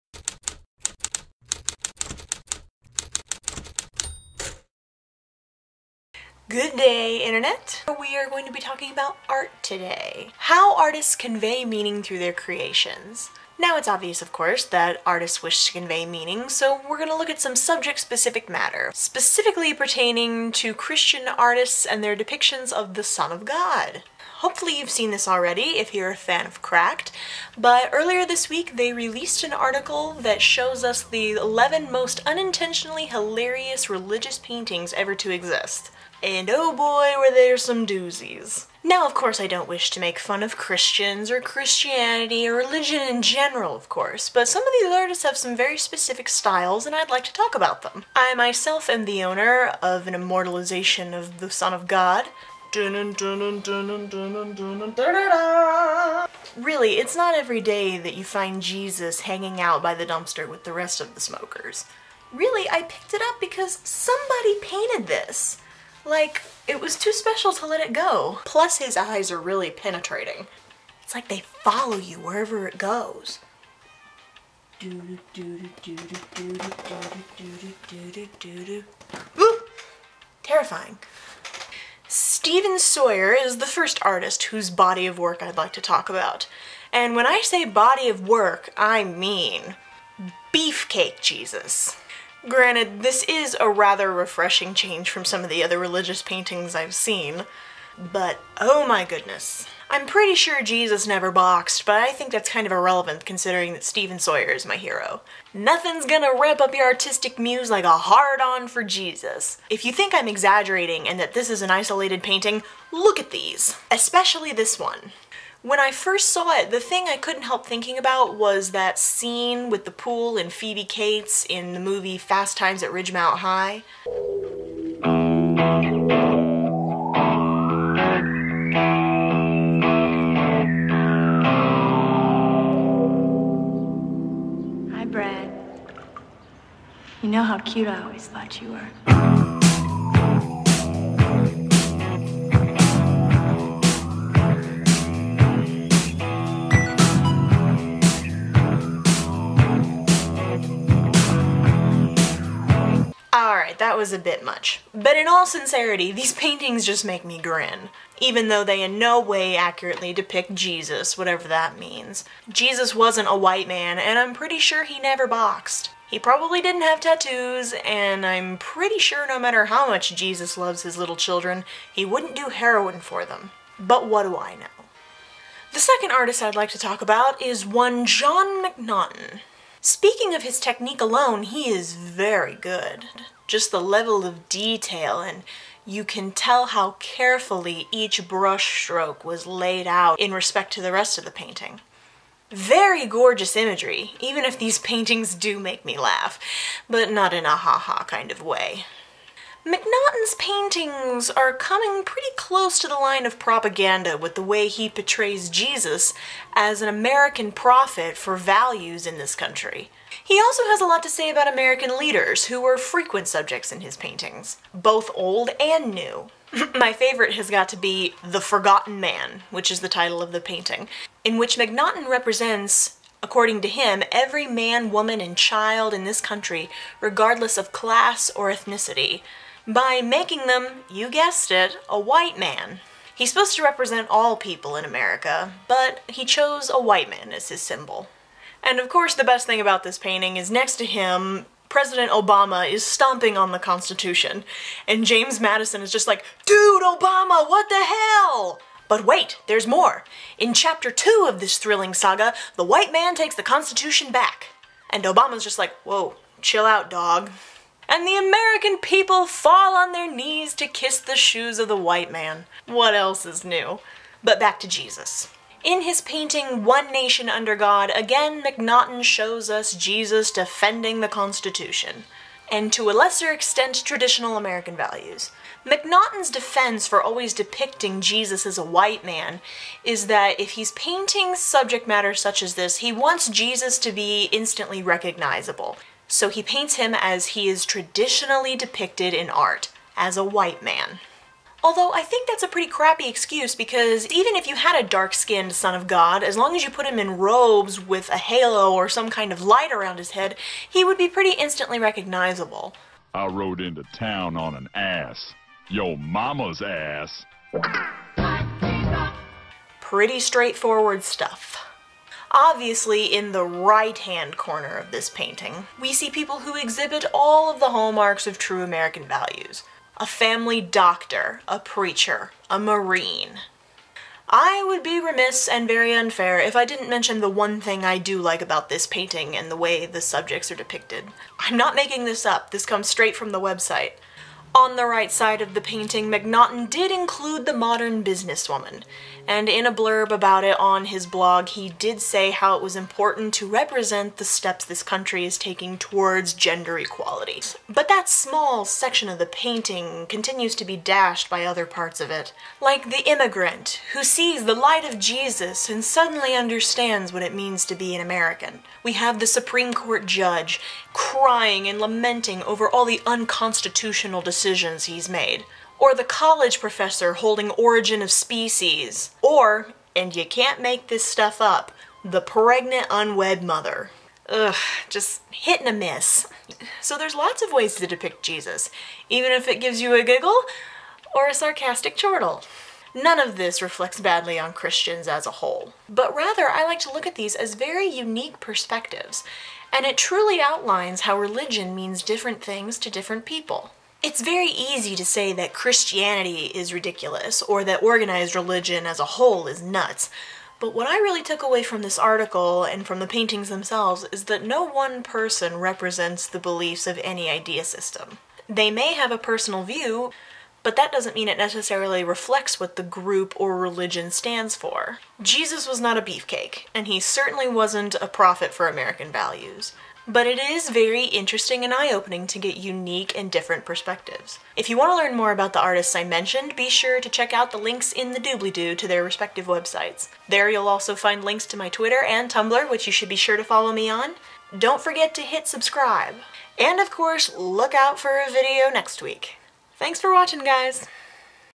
BEEFCAKE JESUS
Today we're talking about some of my favorite contemporary religious paintings.